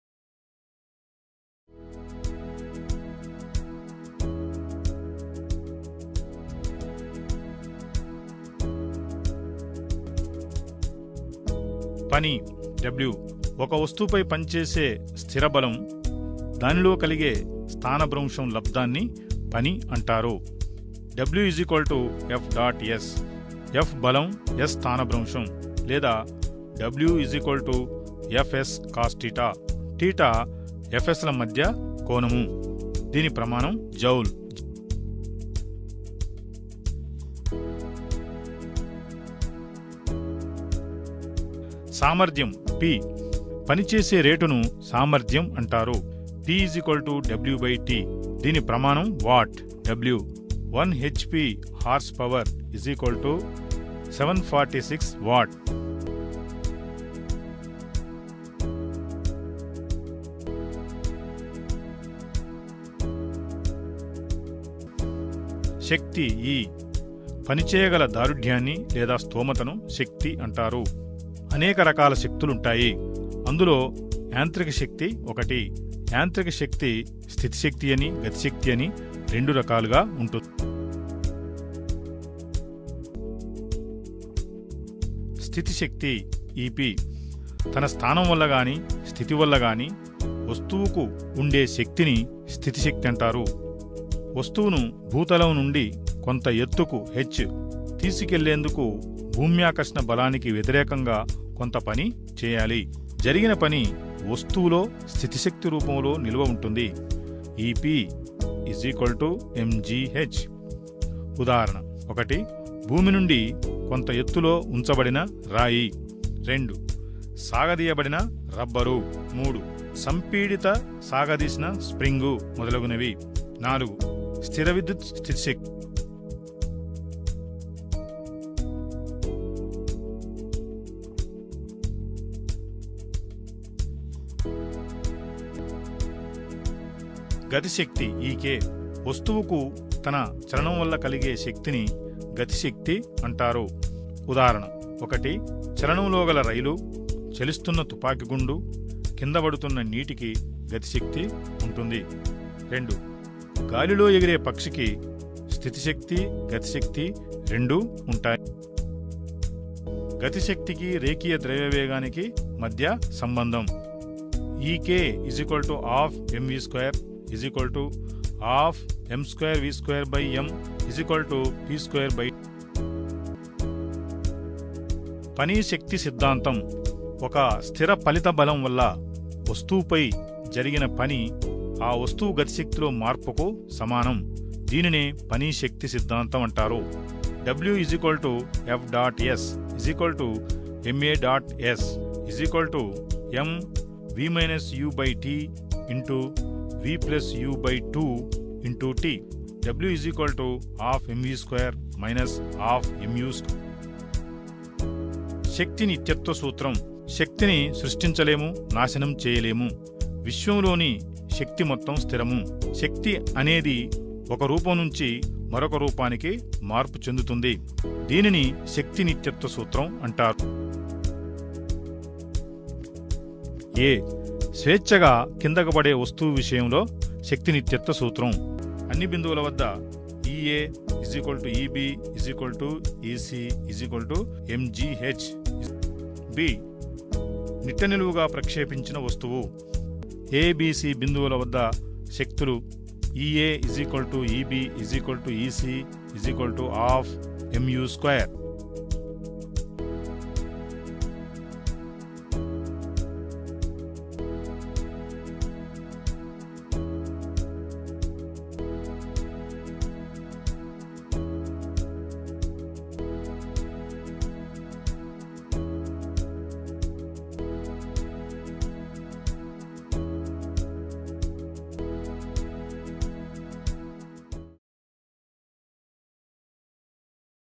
పని, శక్తి - సామర్థ్యం | Work, Energy - Power | Physics Class | Inter 1st Year | Digital Teacher
Today's Topic is Work, Energy - Power, పని, శక్తి - సామర్థ్యం. Inter 1st Year Physics Class In Telugu on Digital Teacher. Digital Teacher is an extraordinary blend of technology and innovation that helps learners to retain the knowledge permanently!